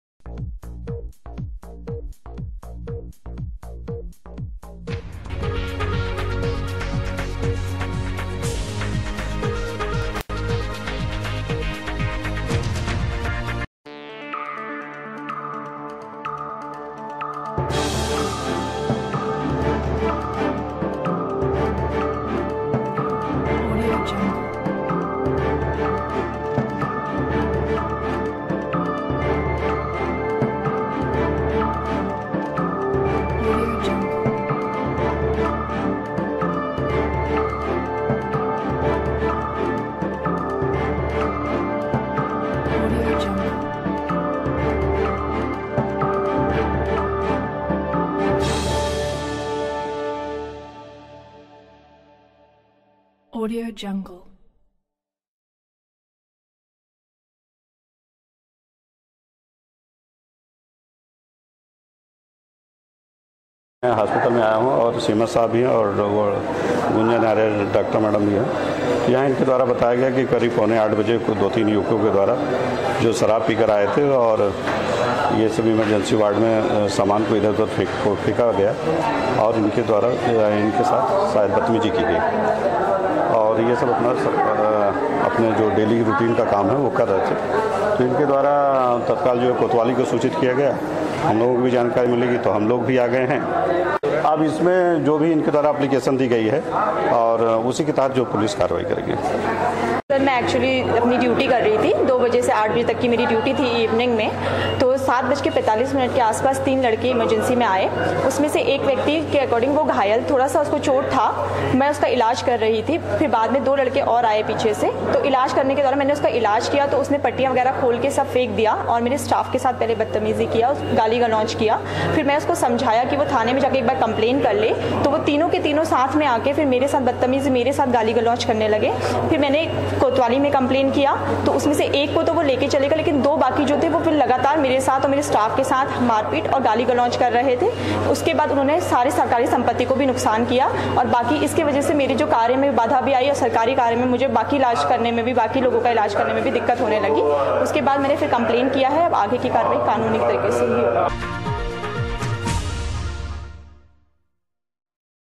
बागेश्वर: जिला अस्पताल में तीन युवकों ने जमकर किया हंगामा, शराब के नशे में की तोड़फोड़
To get news all latest news updates, please follow on our